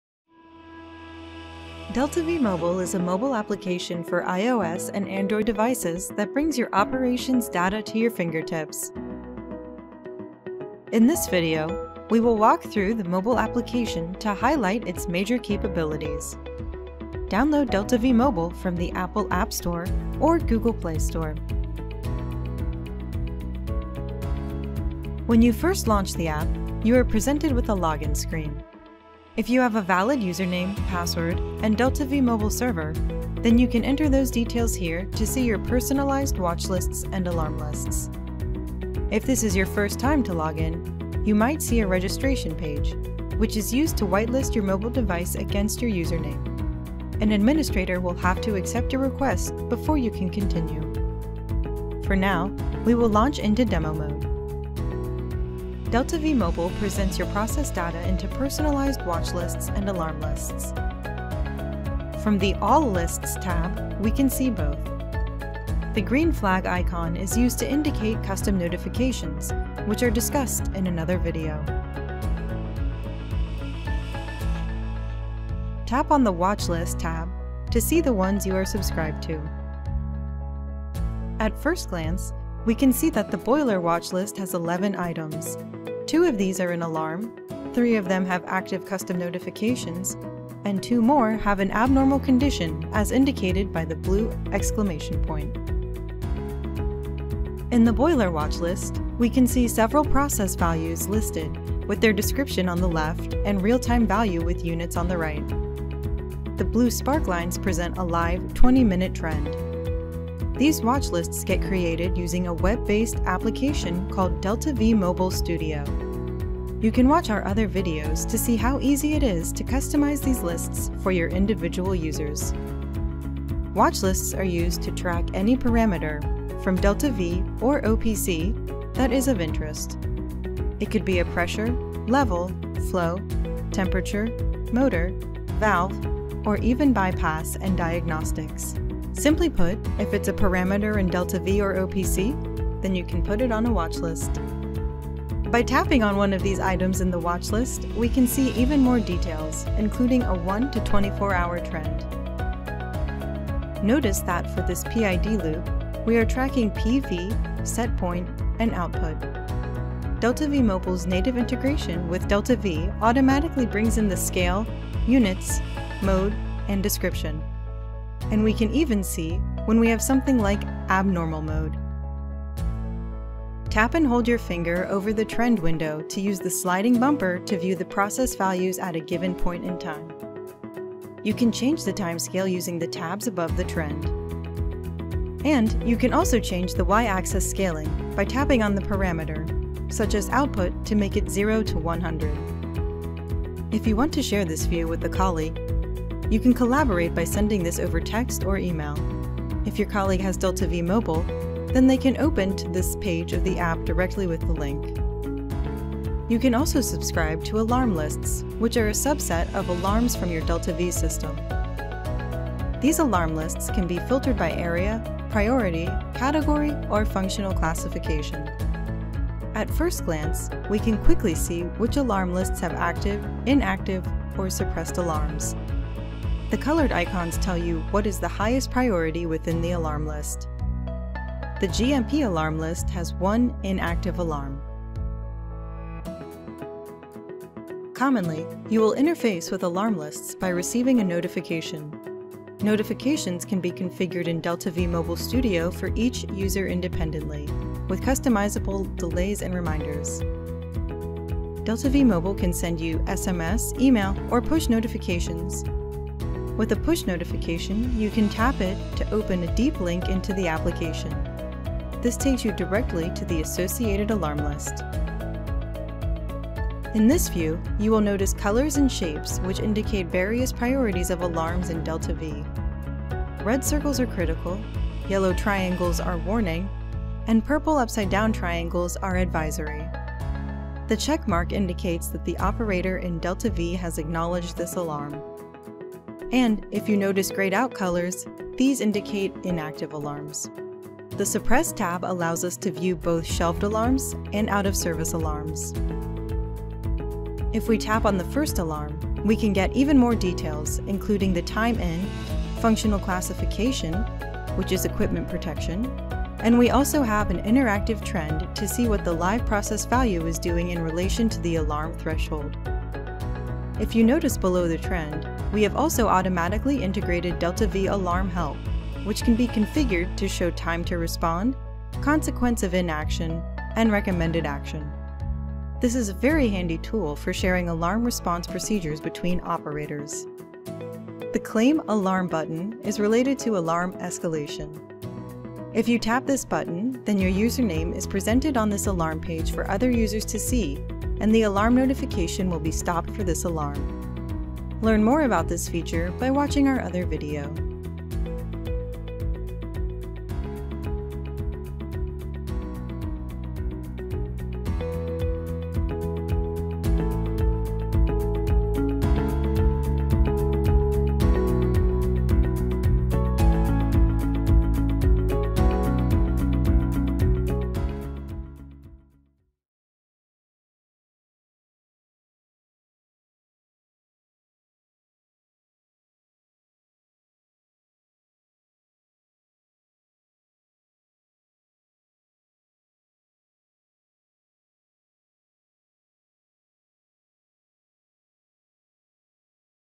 DeltaV Mobile Application Overview
In this video, we will walk through the DeltaV Mobile application to highlight its major capabilities, including data available, watch lists, alarm lists and sharing data with your coworkers.